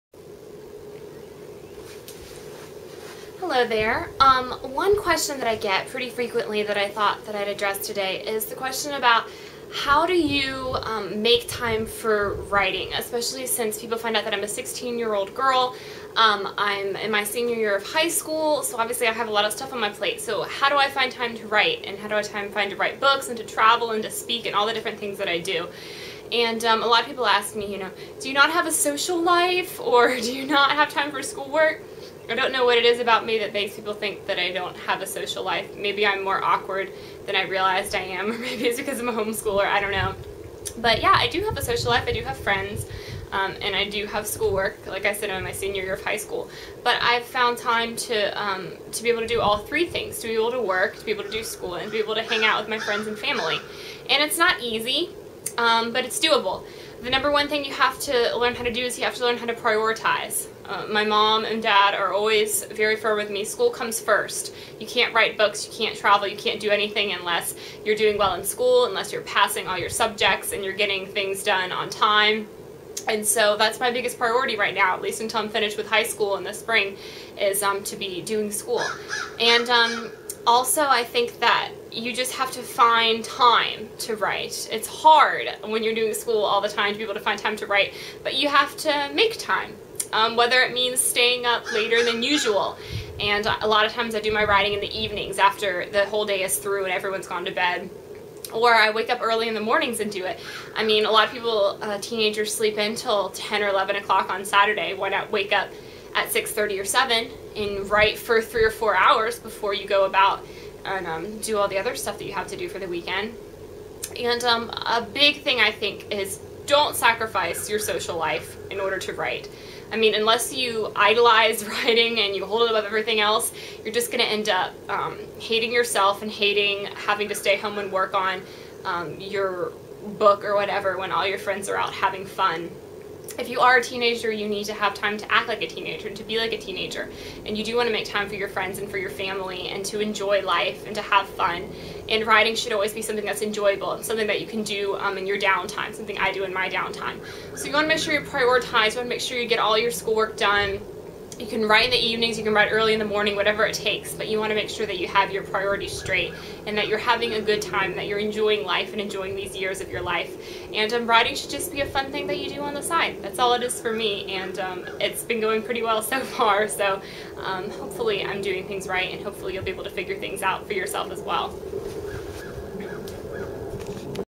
What About Friends and School?
My answer to the ever looming question--"How do you make time for friends and schoolwork"? Some people have a hard time believing that a 16-year-old published author can have a social life. ;)

And order a copy of my debut novel, Interrupted: Life Beyond Words